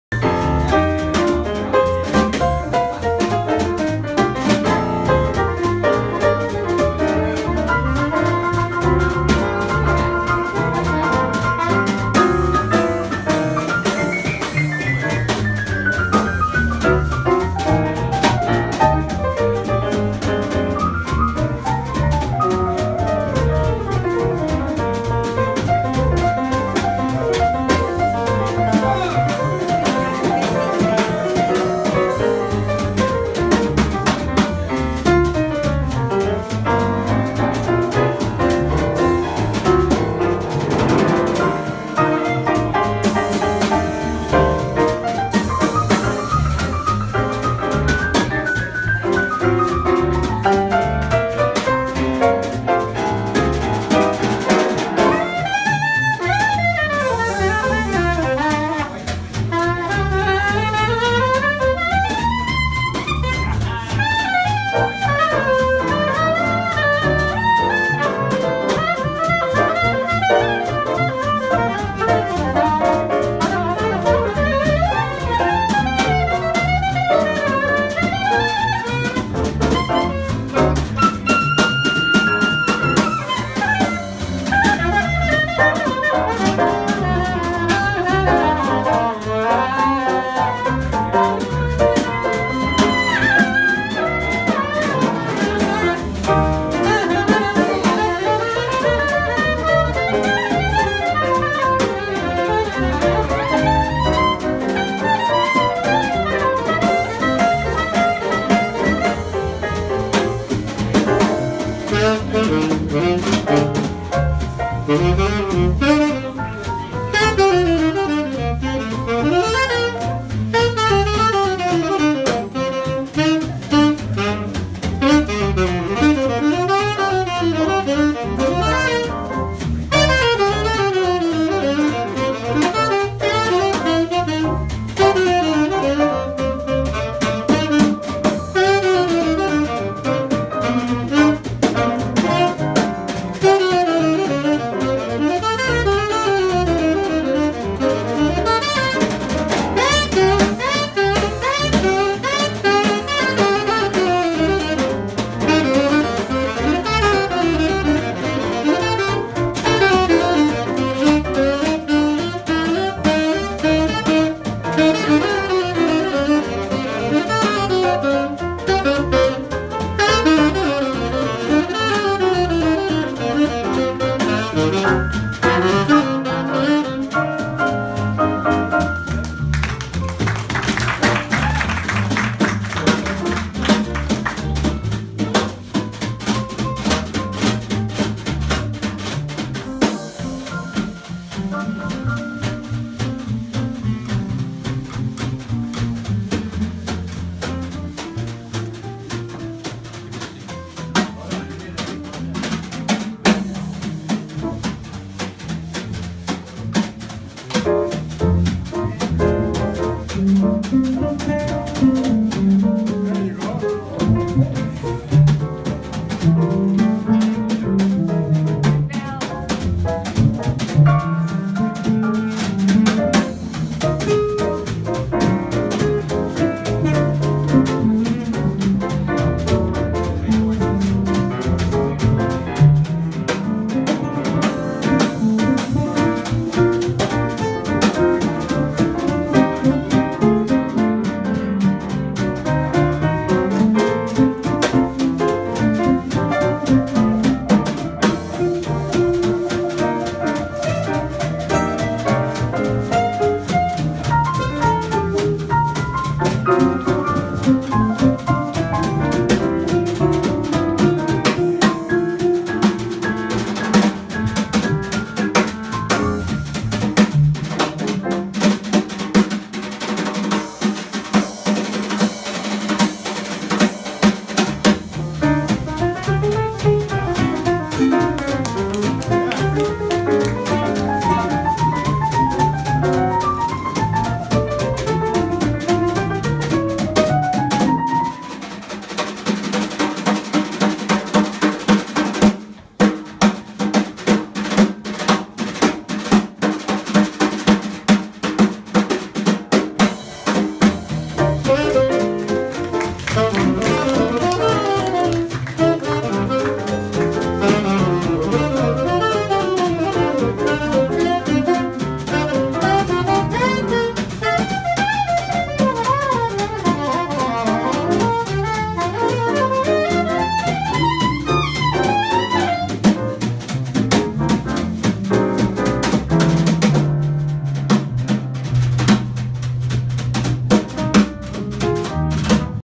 Guillermo Rodriguez playing guitar with Arturo O'Farrill & friends at Afro-Latin Jazz Alliance event at home of Mr. and Ms. Fred Miller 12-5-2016